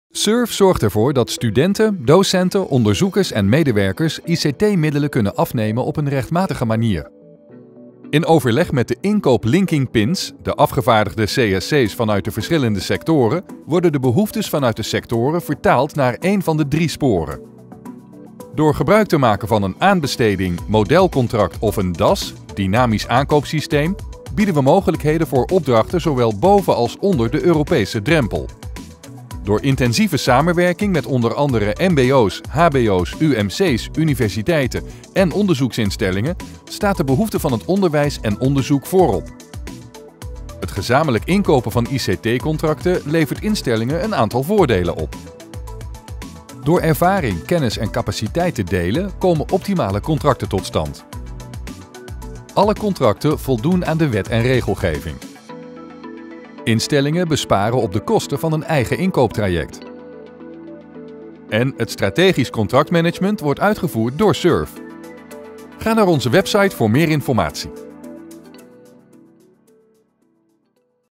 De inkoopmogelijkheden via SURF
SURF biedt onderwijs- en onderzoeksinstellingen drie verschillende inkoopmogelijkheden om ICT middelen op een rechtmatige manier af te nemen. De behoeftes van instellingen worden hierbij vertaald naar een aanbesteding, Model Contact of Dynamisch Aankoopsysteem.